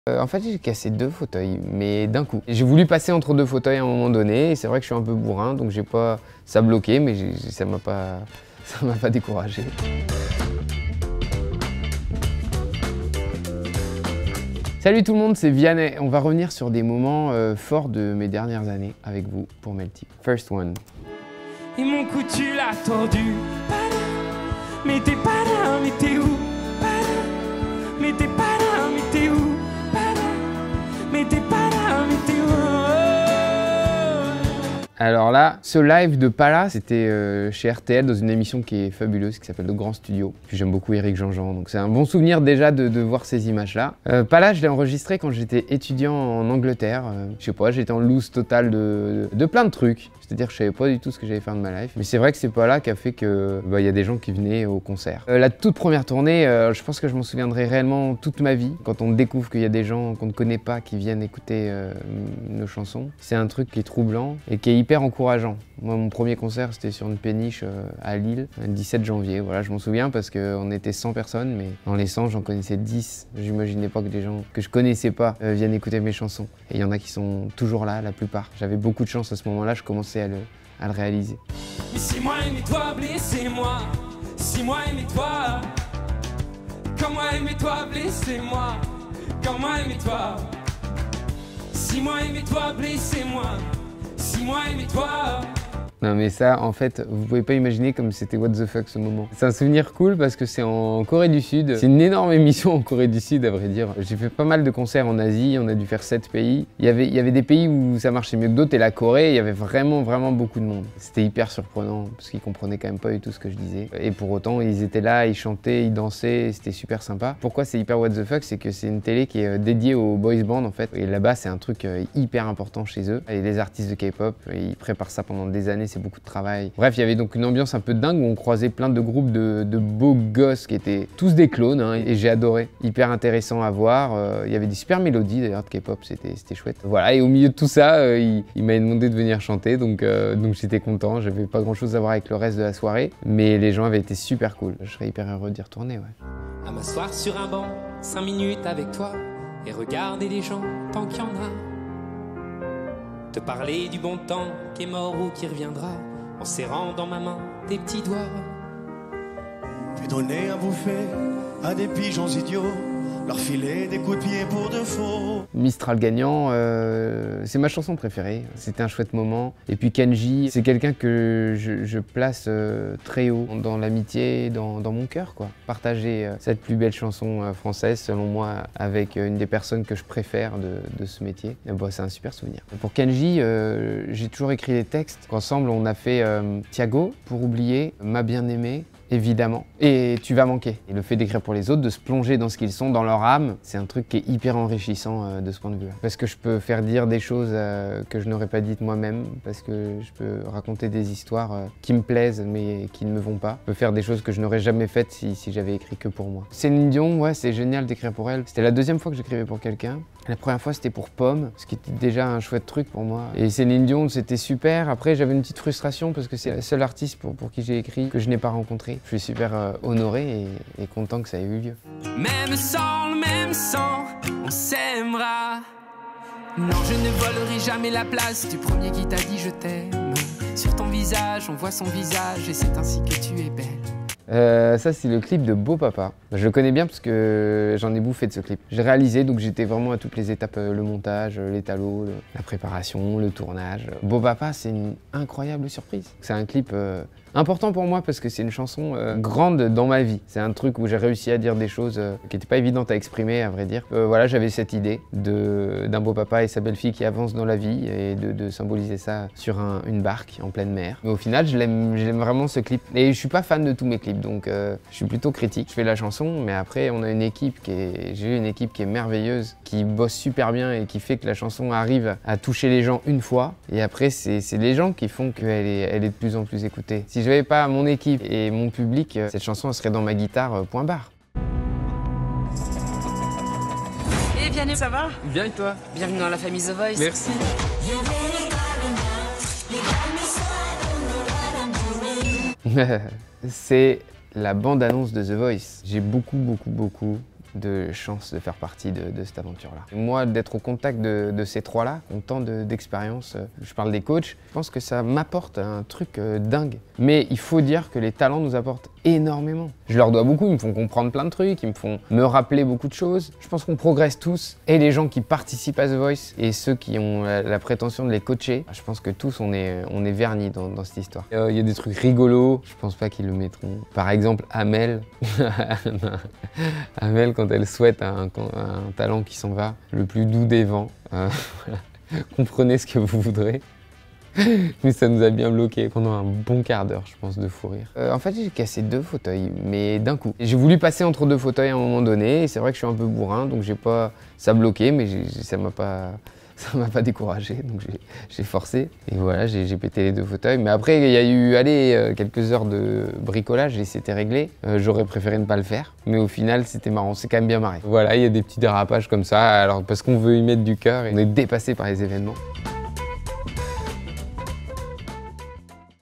Vianney : The Voice, Céline Dion, show improbable en Corée, son amitié avec Kendji (REPLAY)
En détente, Vianney se remate quelques moments forts de sa carrière, d'un show improbable en Corée du Sud à son tout nouveau rôle de juré de The Voice 🎤🪑
Crédits :

►Journaliste : Thomas Meunier
►Réalisation/Montage : Joffrey Couderc